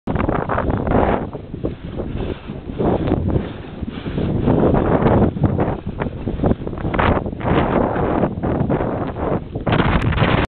Skiing figure eights with Rob
Skiing figure eights with Rob. I love Colorado skiing!